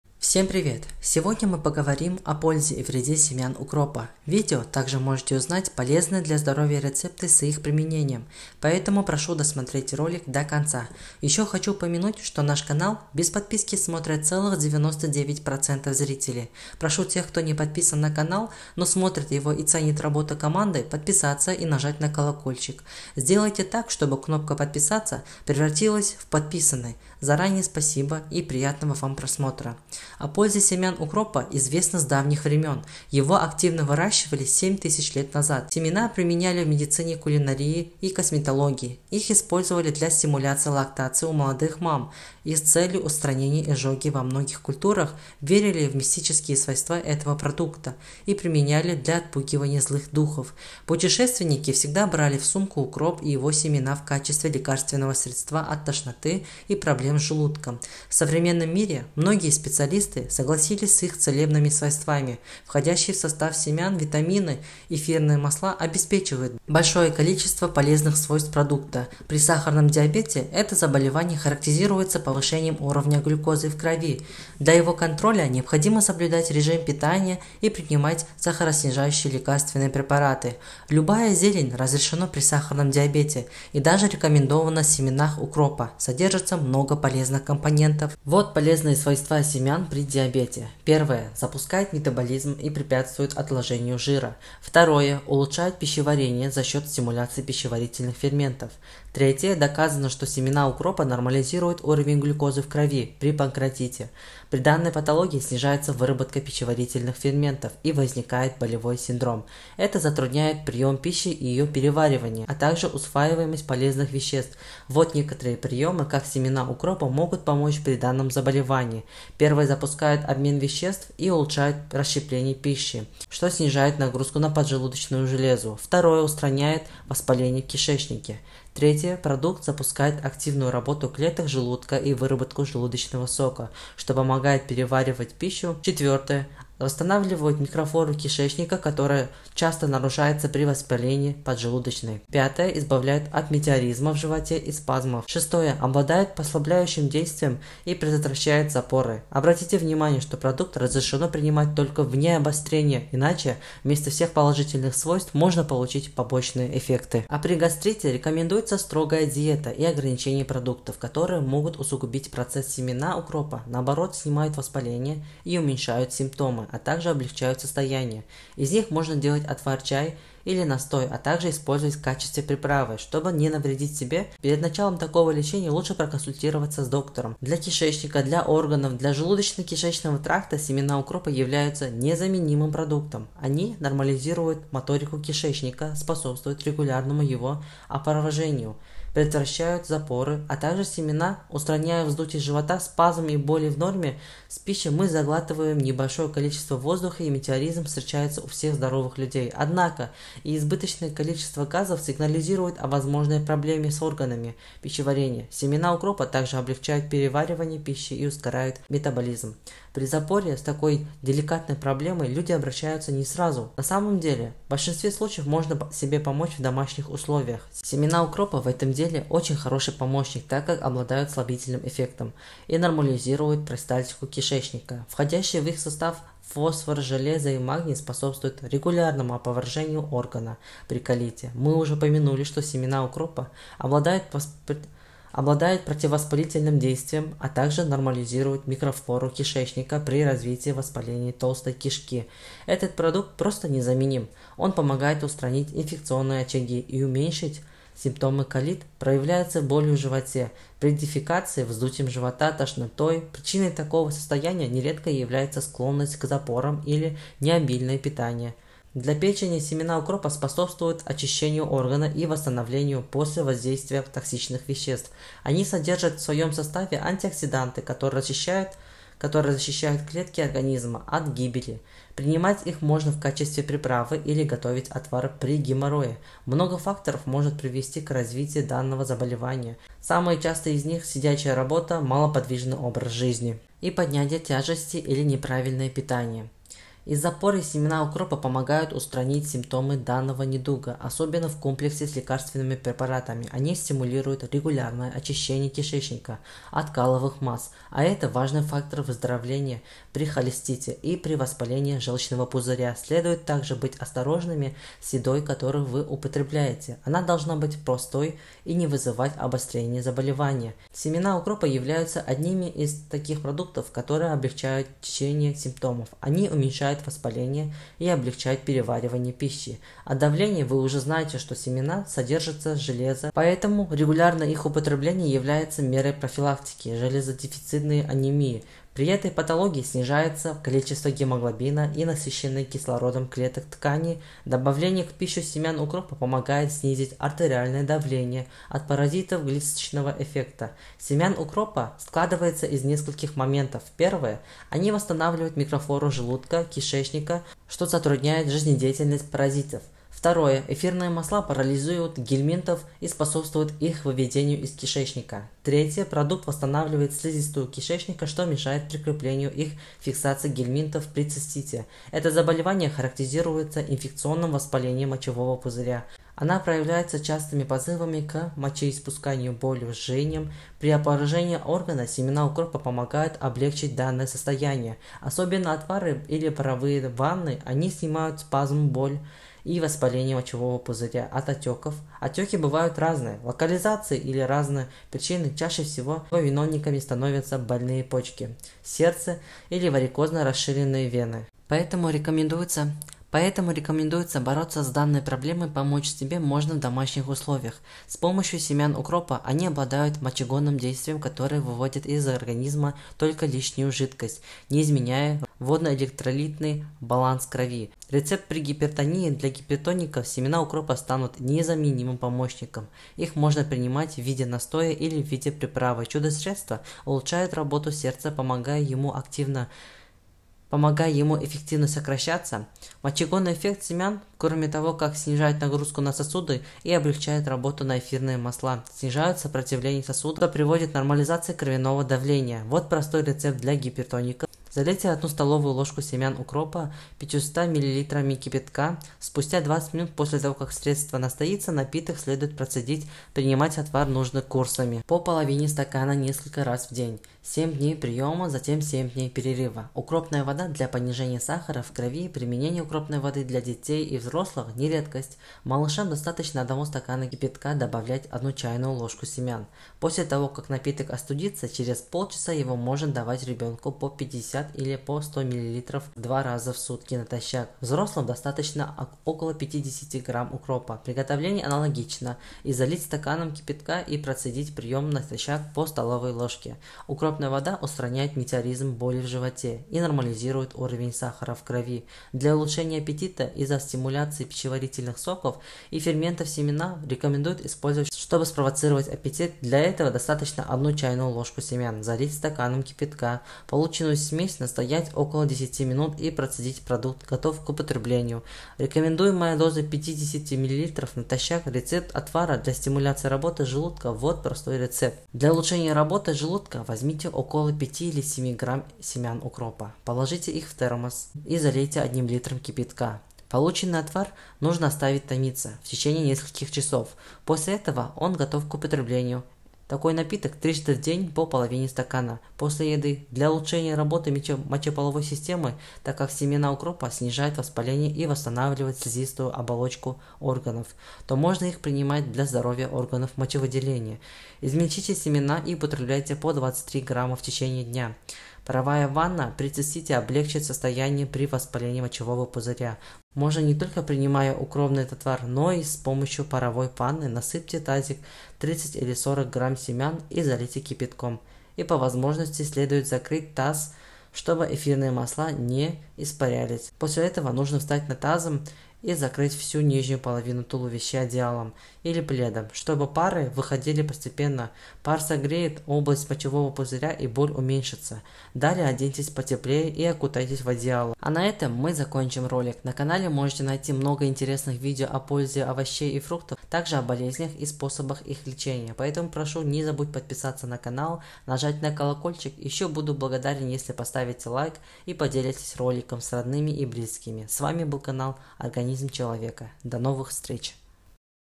Китайский врач: Выпей 100 гр утроит организм как НОВЫЙ. Вы увидите НЕОБРАТИМЫЕ процессы в организме
Что творят семена укропа?
Семена укропа. Какие изменения будут после приема этого способа?

и не забудь включить колокольчик 🔔